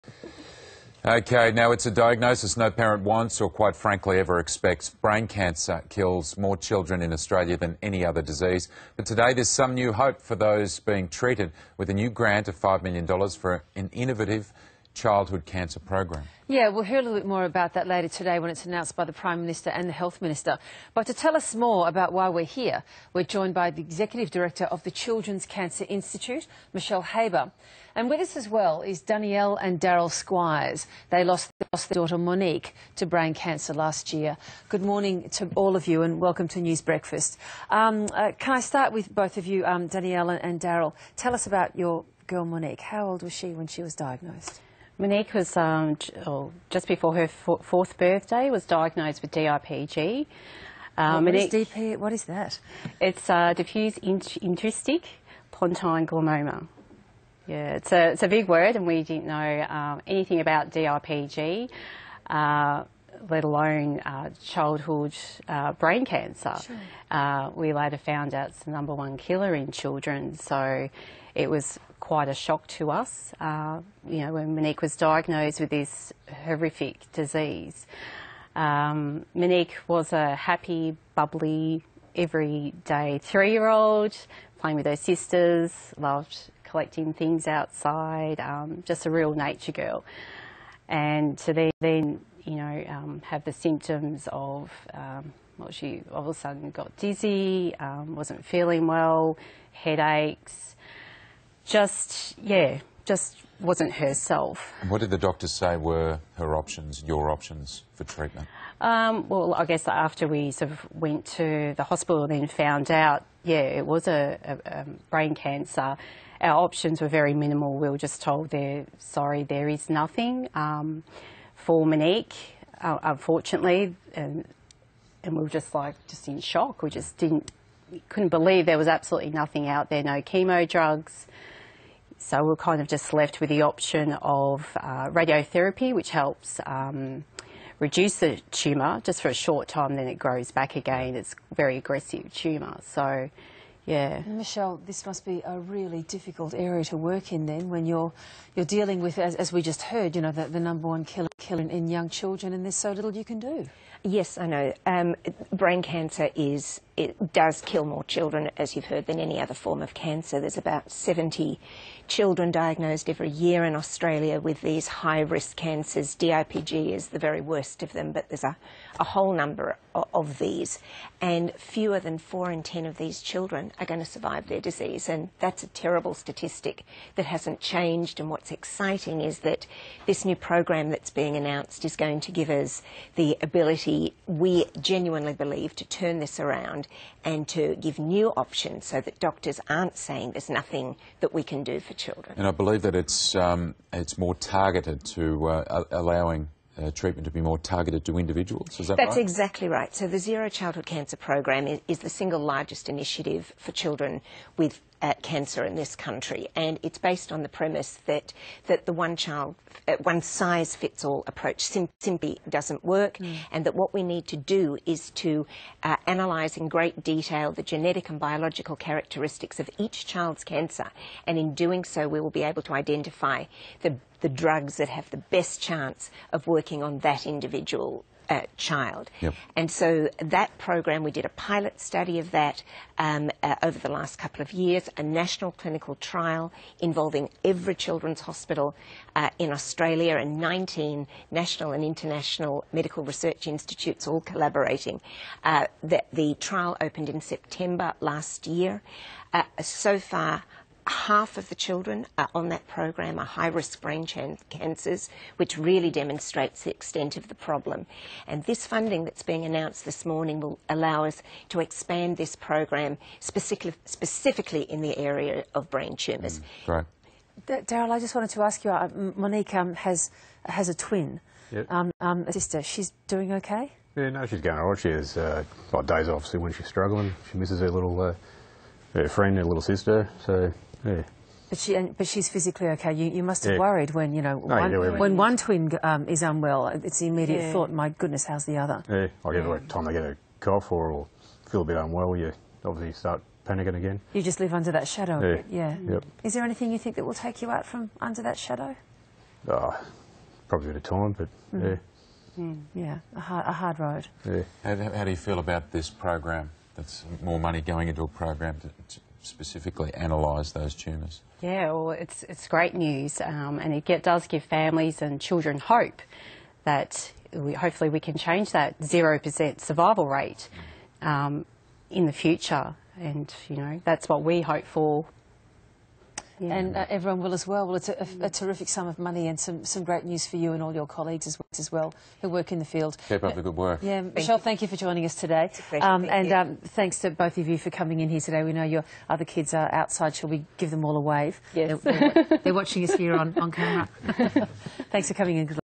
ABC News: Funding announcement to Zero Childhood Cancer with Prof Michelle Haber in the studio
16 July 2018, interview with Prof Michelle Haber and Monique Squires' parents about brain cancer and the announcement of $5 million funding from the Medical Research Future Fund to the Zero Childhood Cancer brain cancer program.